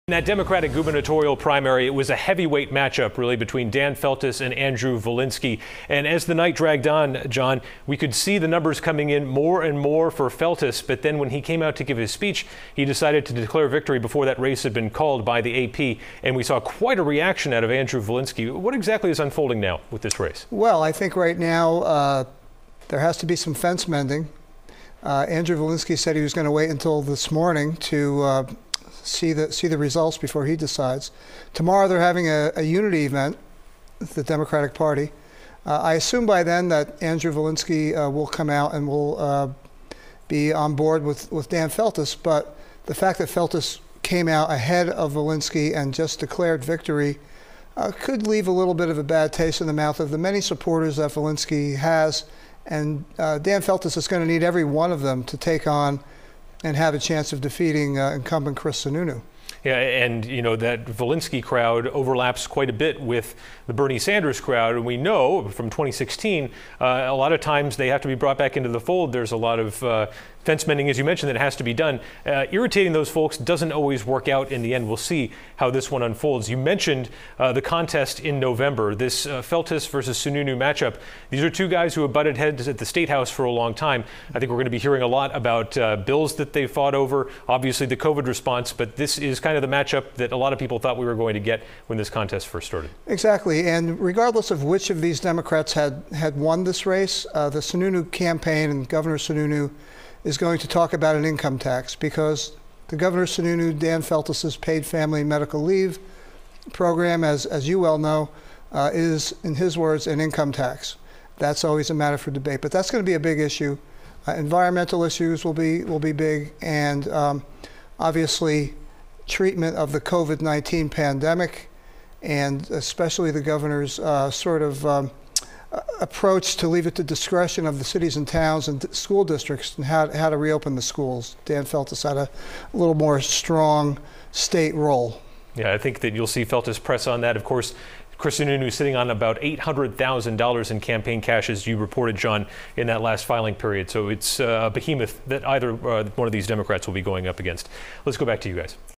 Analysis: Democrat Feltes declares victory in tight gubernatorial primary that's too close to call
Democrat Dan Feltes has declared victory in the Democratic gubernatorial primary, but the race has not been called and Andru Volinsky has not conceded. John DiStaso and Adam Sexton break down the race, and what a Feltes-Sununu matchup looks like for November, if the results hold.